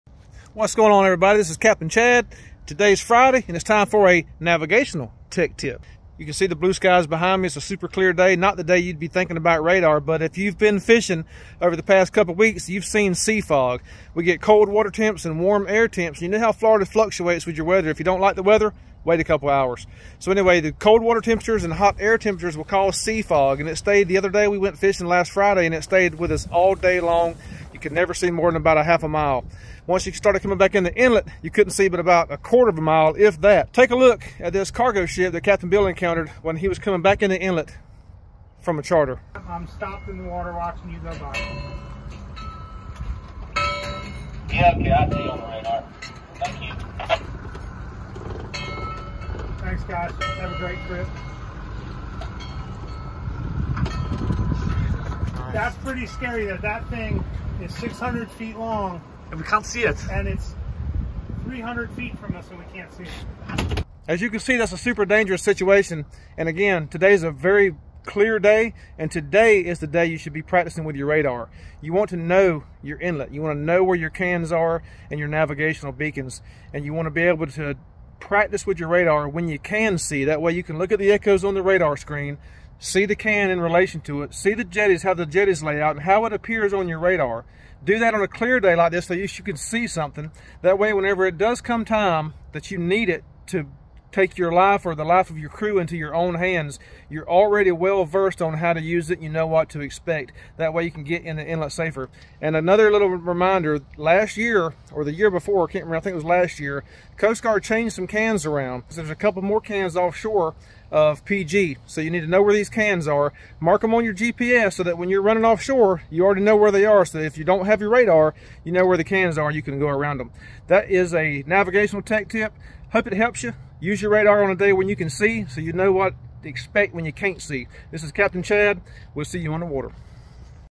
Radar and Sea FOG!!
Practice with your radar when you don't need it, so you won't be blind when you do!